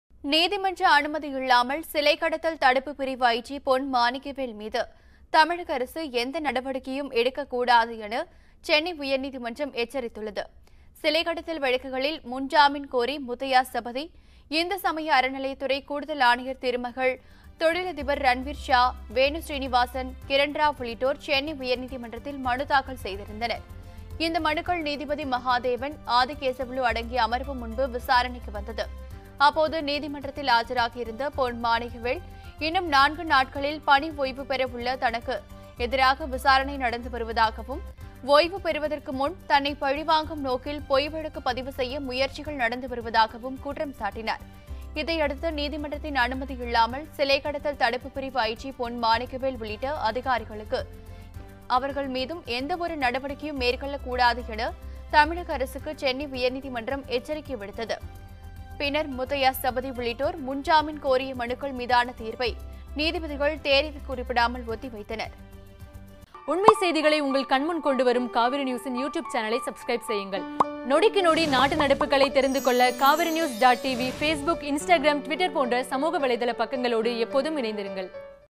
தனக்கு எதிராக விசாரணை நடைபெறுவதாக பொன். மாணிக்கவேல் குற்றச்சாட்டு
பணி ஓய்வுபெறவுள்ள நிலையில் தனக்கு எதிராக விசாரணை நடைபெறுவதாக பொன். மாணிக்கவேல் குற்றச்சாட்டு